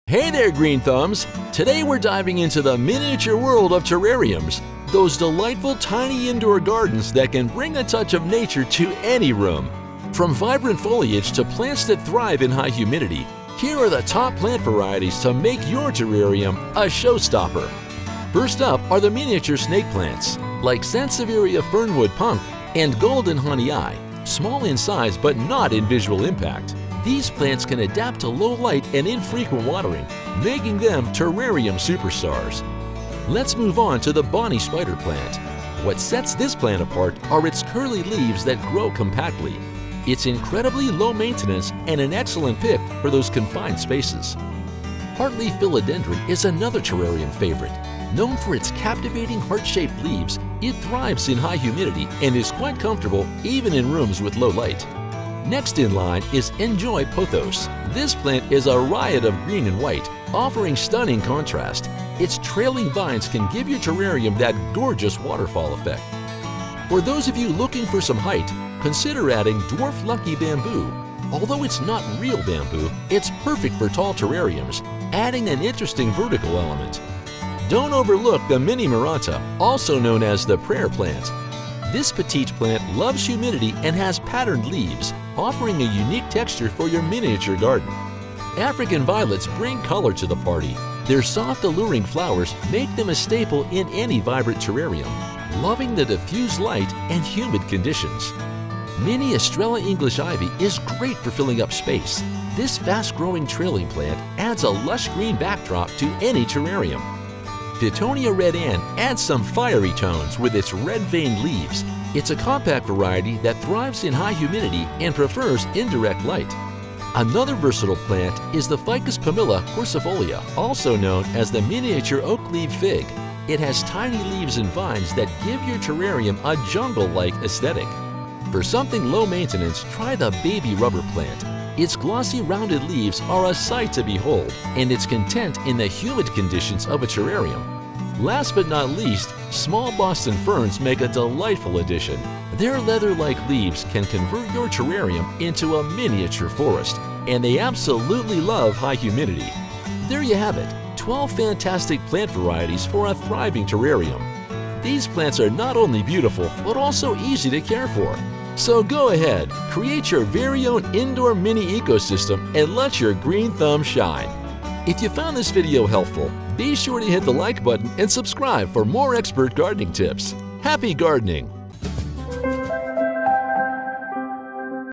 12 Popular Indoor Plant Varieties for Terrariums | Most Reliable Terrarium Plants
Hey there, green thumbs! Today we're diving into the miniature world of terrariums—those delightful, tiny indoor gardens that can bring a touch of nature to any room. From vibrant foliage to plants that thrive in high humidity, here are the top plant varieties to make your terrarium a showstopper.

First up are the Miniature Snake Plants like ‘Sansevieria Fernwood Punk’ and ‘Golden Hahnii’. Small in size but not in visual impact, these plants can adapt to low light and infrequent watering, making them terrarium superstars.

Let's move on to the ‘Bonnie’ Spider Plant. What sets this plant apart are its curly leaves that grow compactly. It's incredibly low-maintenance and an excellent pick for those confined spaces.

Heartleaf Philodendron is another terrarium favorite. Known for its captivating, heart-shaped leaves, it thrives in high humidity and is quite comfortable even in rooms with low light.

Next in line is N’Joy Pothos. This plant is a riot of green and white, offering a stunning contrast. Its trailing vines can give your terrarium that gorgeous waterfall effect.

For those of you looking for some height, consider adding Dwarf Lucky Bamboo. Although it's not real bamboo, it's perfect for tall terrariums, adding an interesting vertical element.

Don’t overlook the Mini Maranta, also known as the Prayer Plant. This petite plant loves humidity and has patterned leaves, offering a unique texture for your miniature garden.

African Violets bring color to the party. Their soft, alluring flowers make them a staple in any vibrant terrarium, loving the diffused light and humid conditions.

Mini Estrella English Ivy is great for filling up space. This fast-growing, trailing plant adds a lush green backdrop to any terrarium.

Fittonia ‘Red Anne’ adds some fiery tones with its red-veined leaves. It's a compact variety that thrives in high humidity and prefers indirect light.

Another versatile plant is the Ficus pumila ‘Quercifolia’, also known as the Miniature Oakleaf Fig. It has tiny leaves and vines that give your terrarium a jungle-like aesthetic.

For something low-maintenance, try the Baby Rubber Plant. Its glossy, rounded leaves are a sight to behold, and it's content in the humid conditions of a terrarium.

Last but not least, Small Boston Ferns make a delightful addition. Their feather-like leaves can convert your terrarium into a miniature forest, and they absolutely love high humidity.

There you have it—12 fantastic plant varieties for a thriving terrarium. These plants are not only beautiful but also easy to care for. So go ahead, create your very own indoor mini-ecosystem and let your green thumb shine.